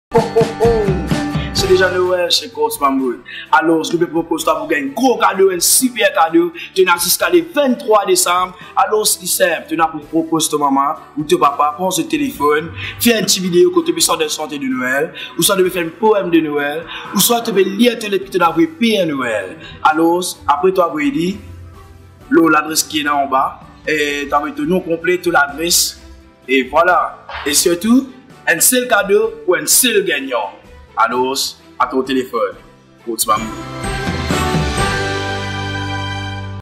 🎅NOËL EST DÉJÀ LÀ CHEZ COURTS MAMMOUTH ft. BIGG FRANKII⠀
C’est bientôt Noël !🎄⠀
La période de Noël est synonyme de cadeaux ! Notre ami Bigg Frankii et Courts Mammouth l'ont bien compris !⠀
Afin de vous aider à passer un Noël des plus festifs, Courts Mammouth lance son concours ce jeudi 12 décembre.⠀
🎁Son principe est simple : Poème de Noël, lettre au Père Noël ou chant de Noël, à vous de choisir ! Si vous avez entre 2 et 12 ans, il suffit de demander la permission à vos parents et de leur emprunter leur téléphone pour réaliser une petite vidéo de 15 secondes où vous nous lirez votre lettre, réciterez votre poème et chanterez votre chanson ! ⠀
🎁Vous aurez jusqu'au 23 décembre pour nous l'envoyer à l'adresse suivante : ⠀
🎁Attention : Un seul gagnant partira avec un grand cadeau !⠀
Un tirage au sort sera effectué et le gagnant sera contacté par téléphone.⠀
Préparez-vous, ça vient, et ça vient vite ! Cette année, le Père Noël chante avec Bigg Frankii pour vous couvrir de cadeaux !🎅⠀